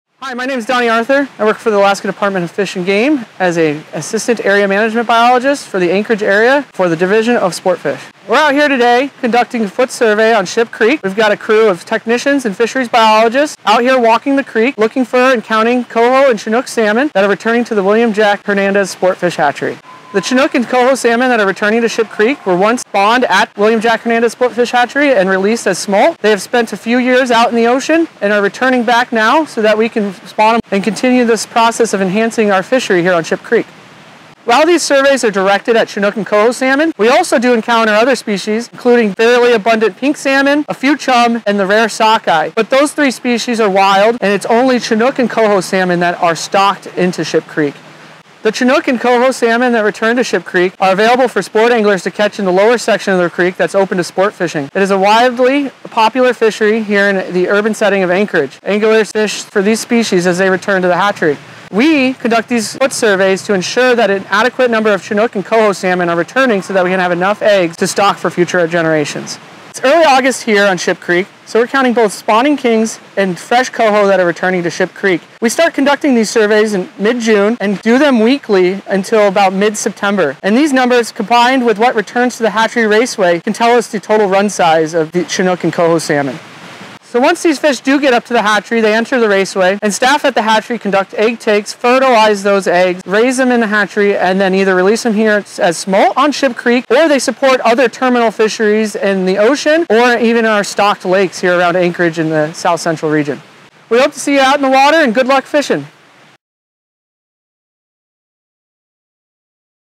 Counting Salmon in the heart of Anchorage - Ship Creek Stream Survey
Join fisheries biologist Donnie Arthur on a stream survey of Ship Creek in the heart of Anchorage, AK. ©ADF&G